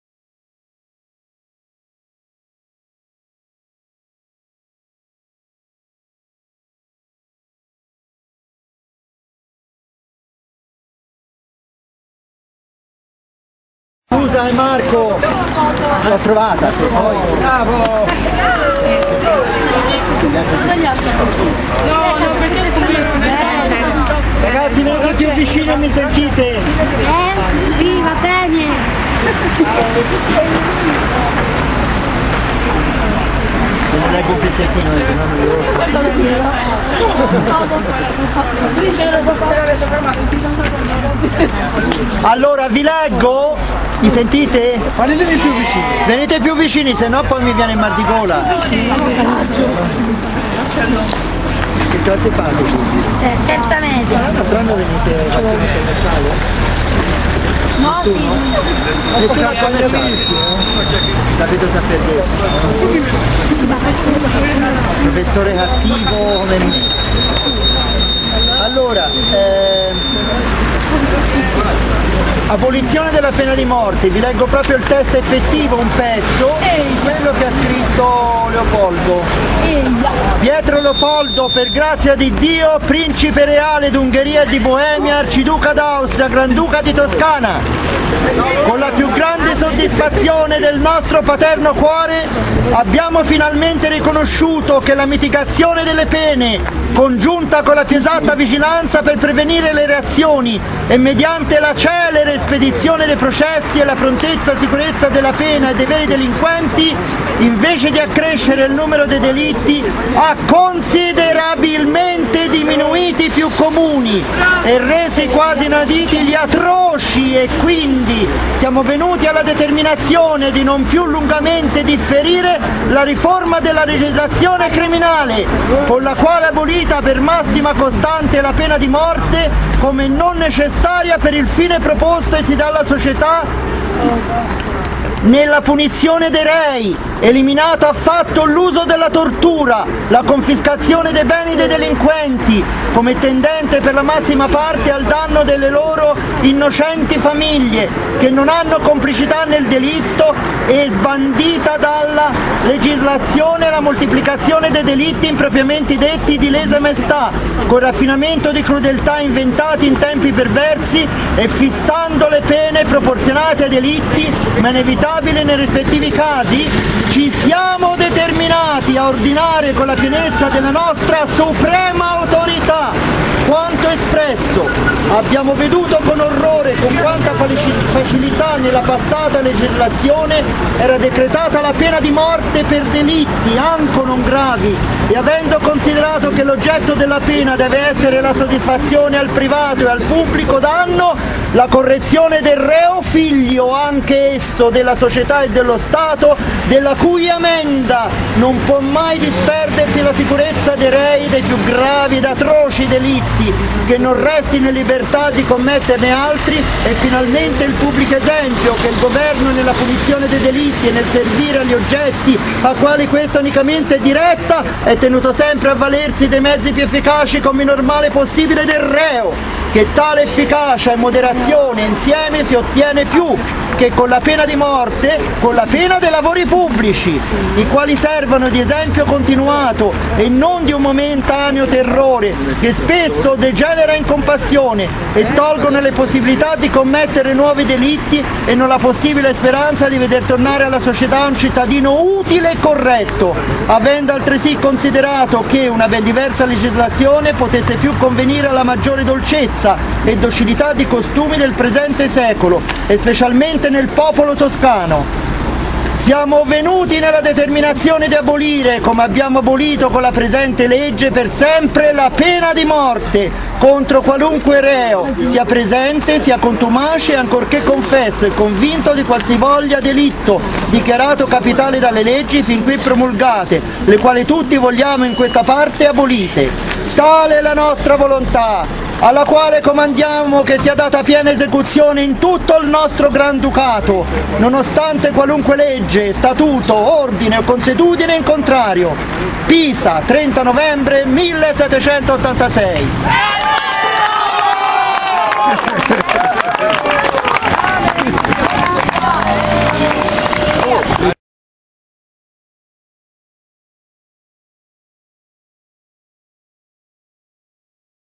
abolizione della pena di morte
Abolizione della pena di morte .Era il 30 Novembre 1796 Pietro Leopoldo gran duca di Toscana  è stato il primo nel mondo ad abolire la pena di morte. nel filmato amatoriale il Prof. Maurizio Righi agli studenti  legge il proclama.